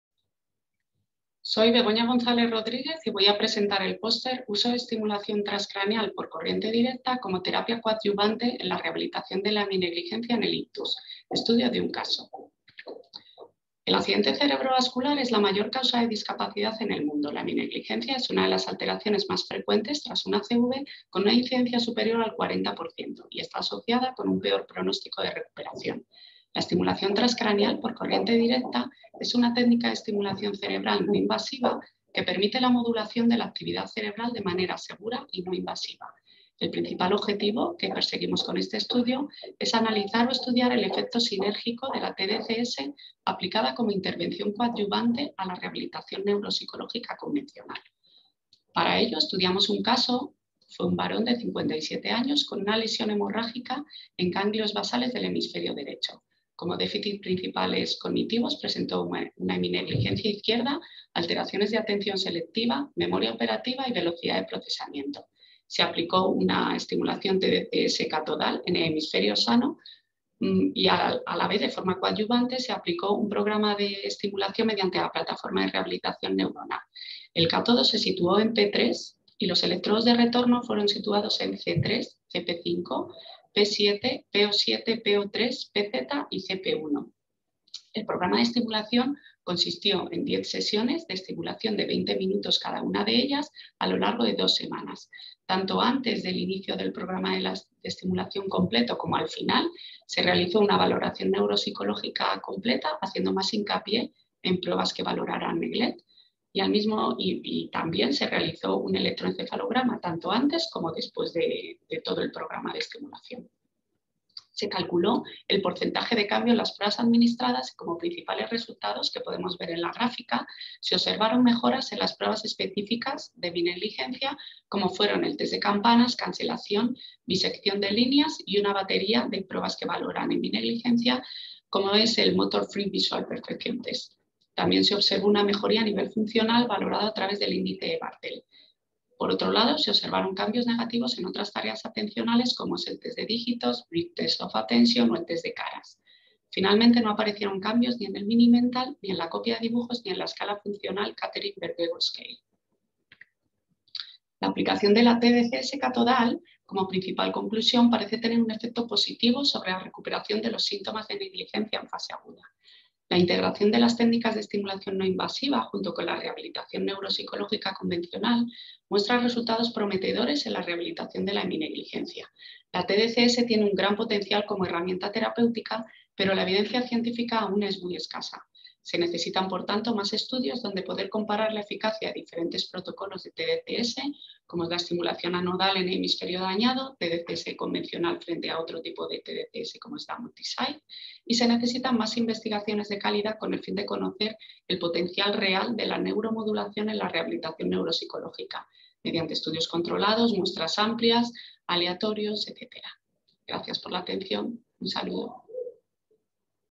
Resultados de nuestro estudio de tratamiento de la heminegligencia mediante estimulación eléctrica
El objetivo de la investigación – de la que Begoña González ha presentado el estudio de un caso en esta XIII edición del Congreso FANPSE- es estudiar el efecto sinérgico de la tDCS aplicada como intervención coadyuvante a la rehabilitación neuropsicológica convencional.

Más info